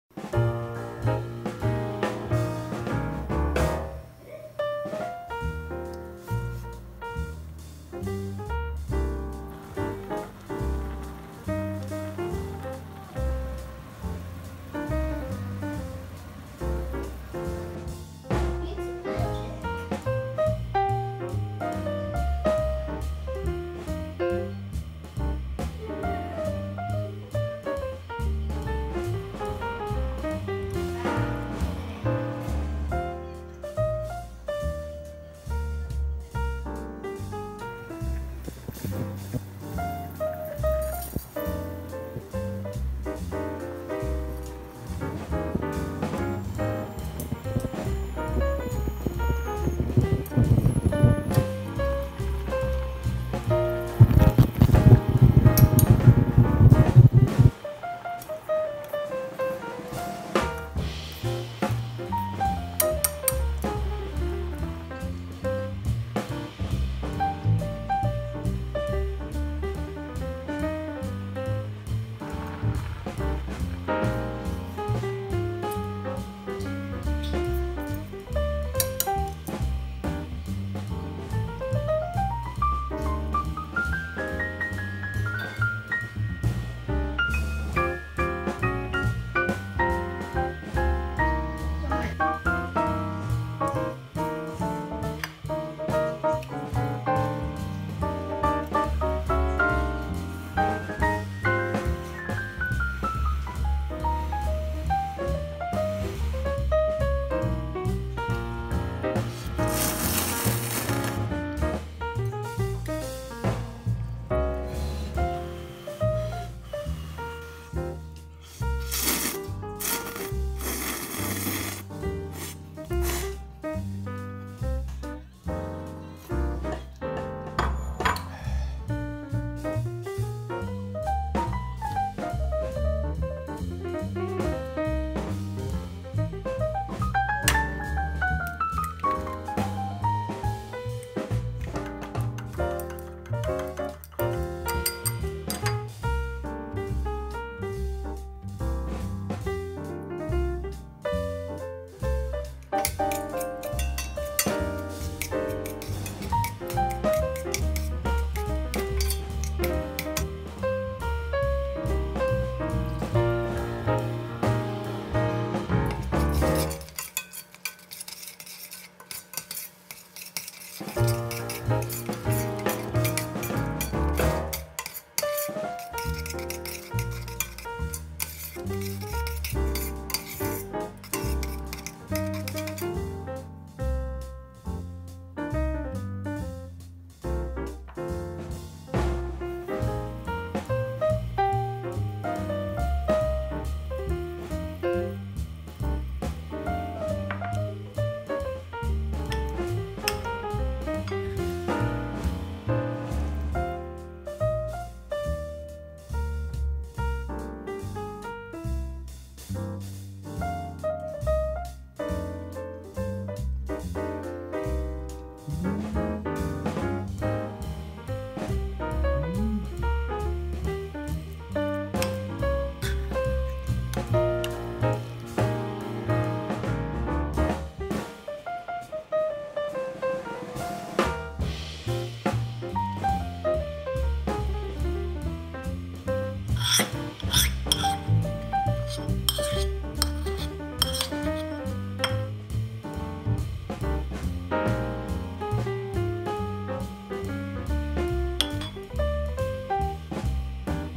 라면국물을 활용한 완벽한 1+3 식사.
라면을 맛있게 먹는 방법은 많지만 항상 새로운 방법이 나타나고 있습니다. 이번 동영상에서도 라면의 면빨은 그저 거들뿐...라면국물로 새로운 요리를 선보였지만 앞으로 얼마나 더 새로운 요리가 탄생할지 기대해주세요.